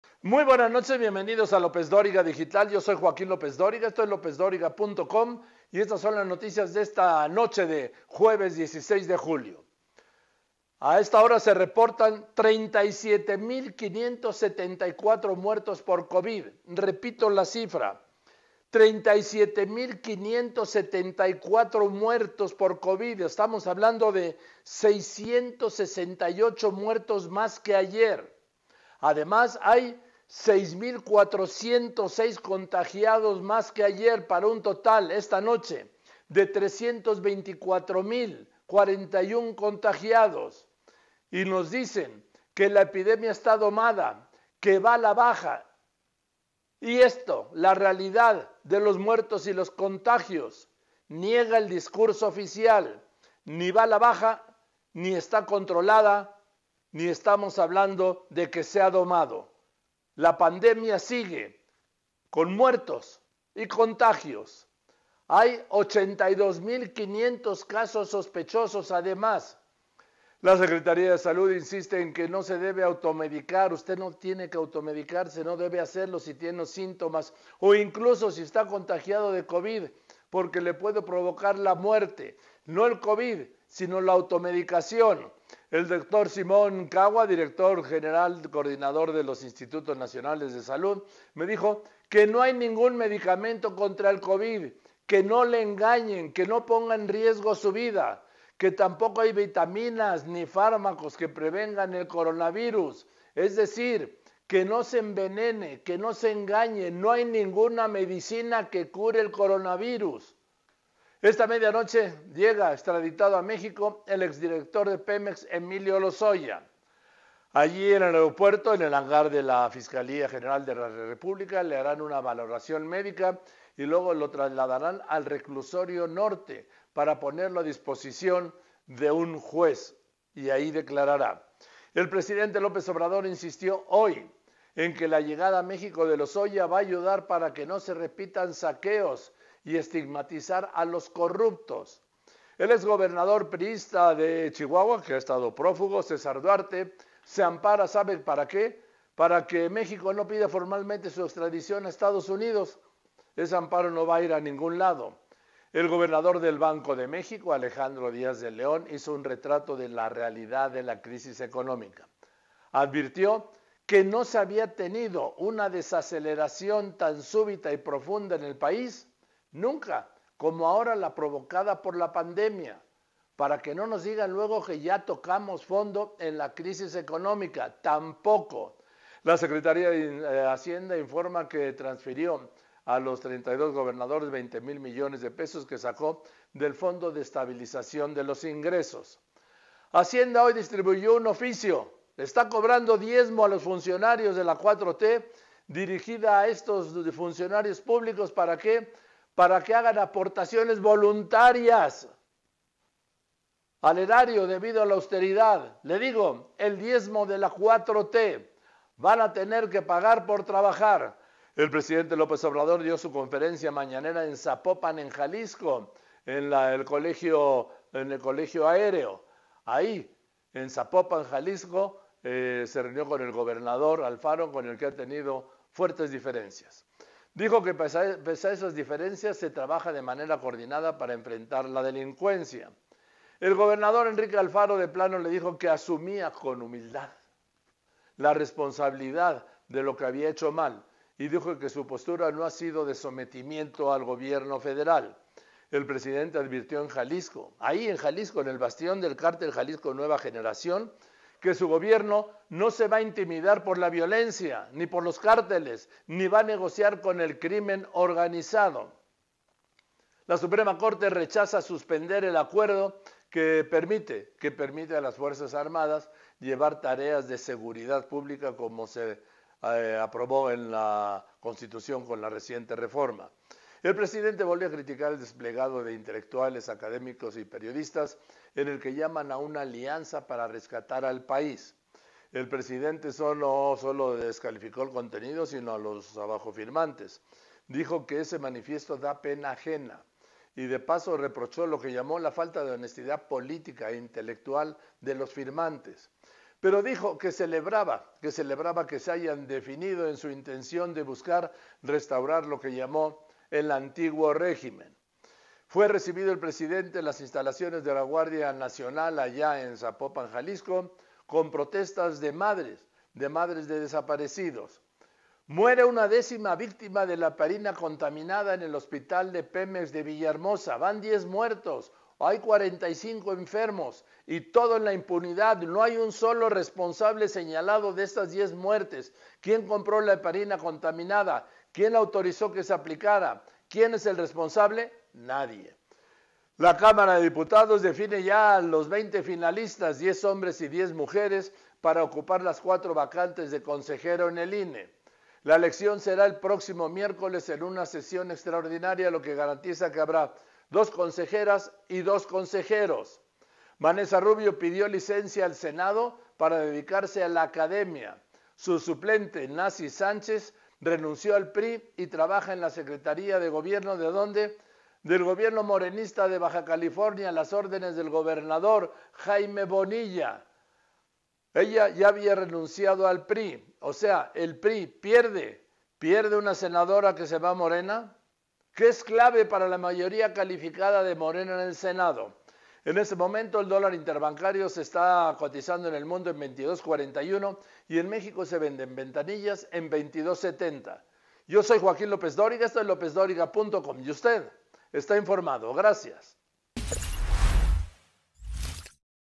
¡Las noticias! Banxico advierte de desaceleración súbita y acelerada por la pandemia
¡Las noticias!
No hay ningún medicamento, ni vitaminas ni fármacos que cure el coronavirus; no tarda en llegar Emilio Lozoya a México; el gobernador de Banxico advirtió que no había una desaceleración tan súbita y profunda en el país como la provocada por la pandemia y más